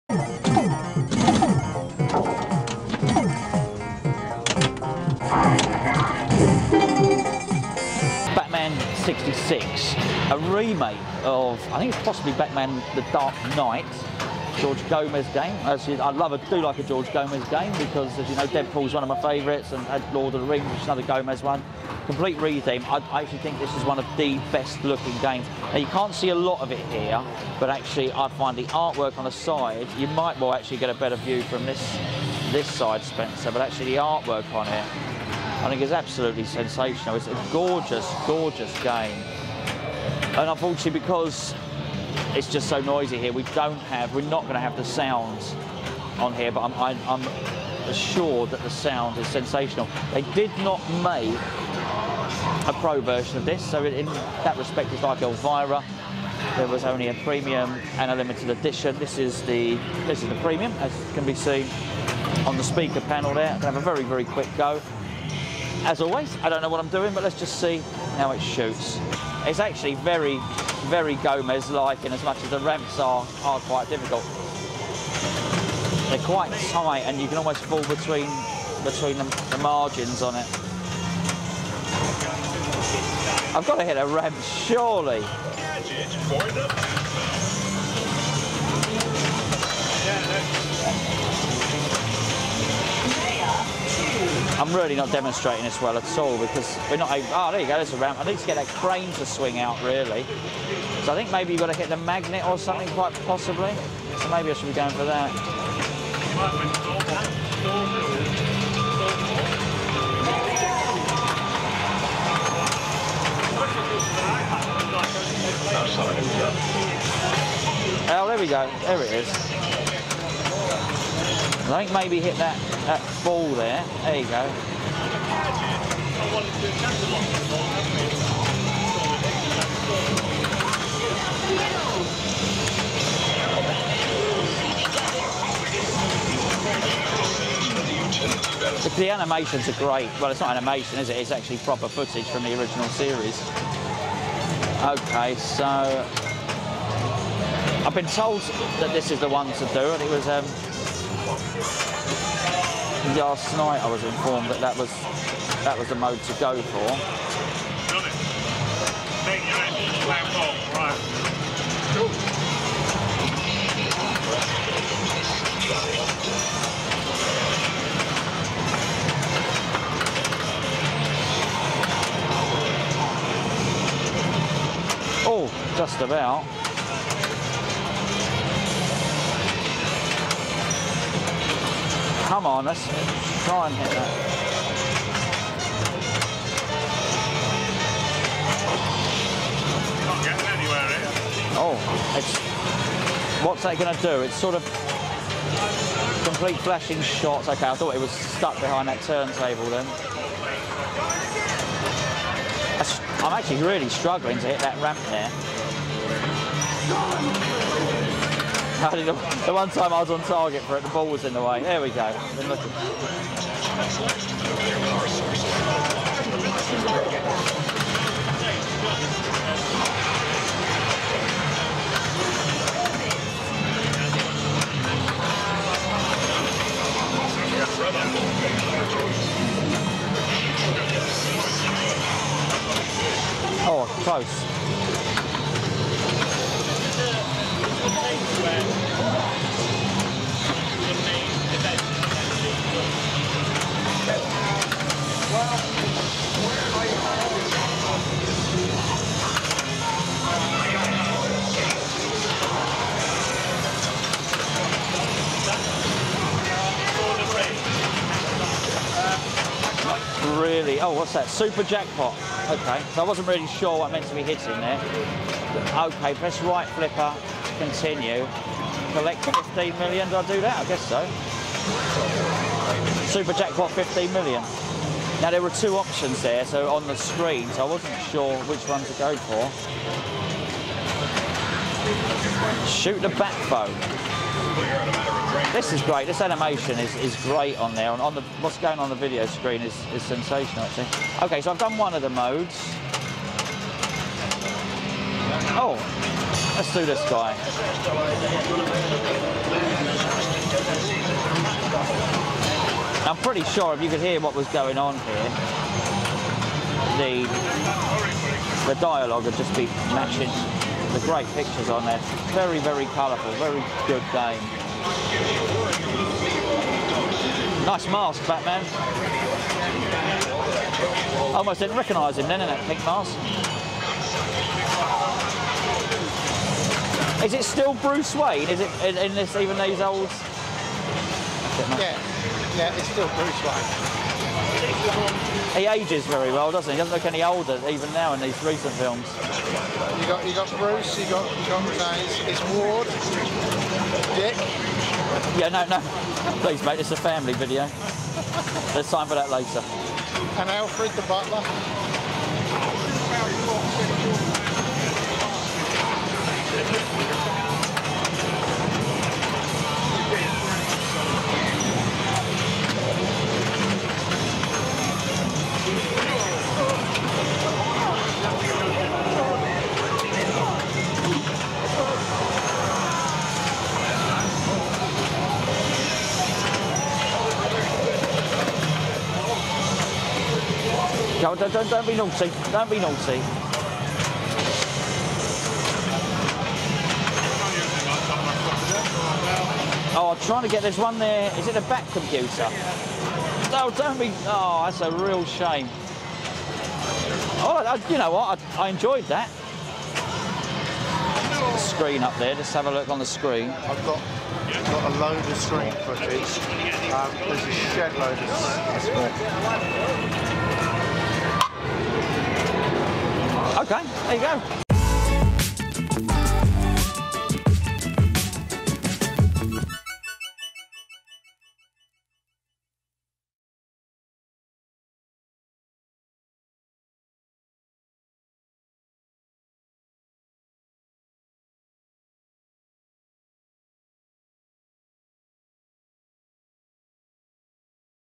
Stern Batman 66 Pinball Machine | A quick look at the playfield, cabinet & gameplay
Gonzo & Spencer take a first look at Sterns Batman 66 by designer George Gomez